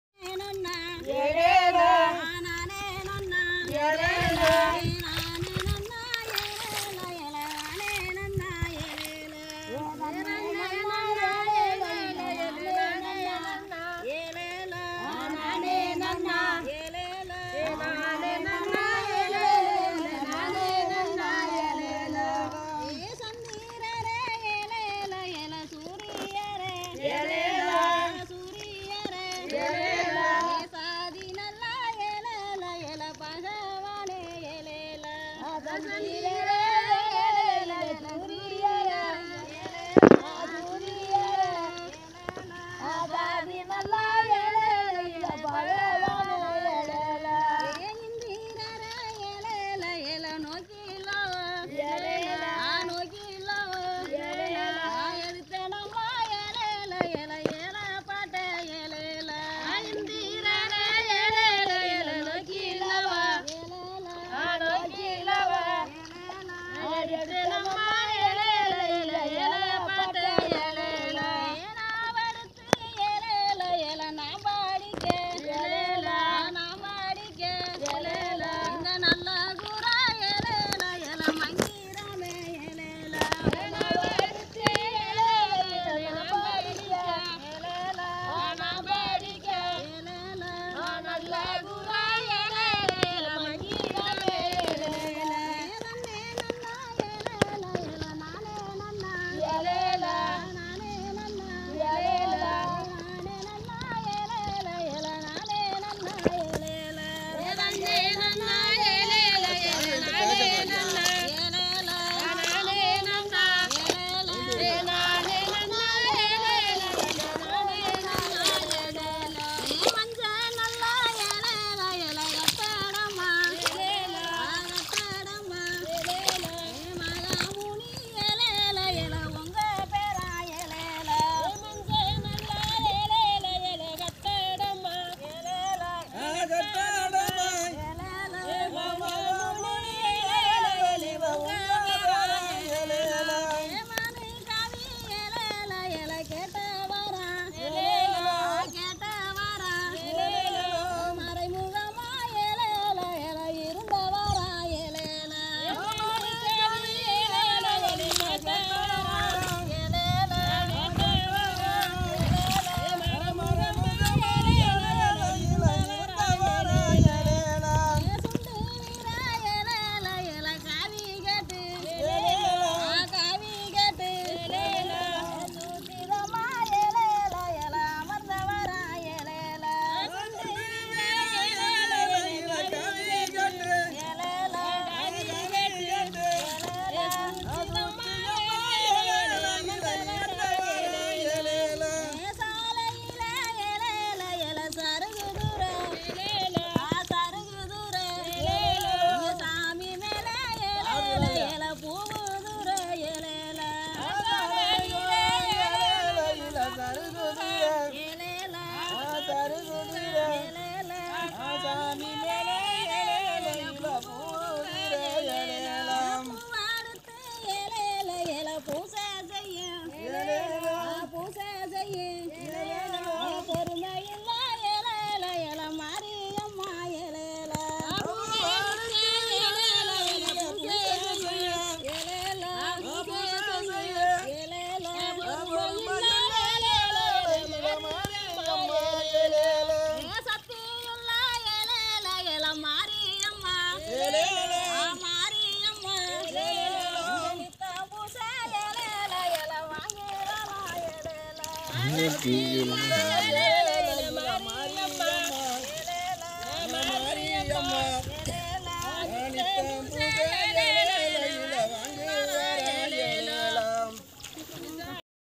மணப்பாறை பக்கத்தில் f.கீழையூர் கிராமம் மணப்பட்யில் நாத்து நடும்போது பாட்டு பாடிய அருமையான காட்சி
பாடகி-சா.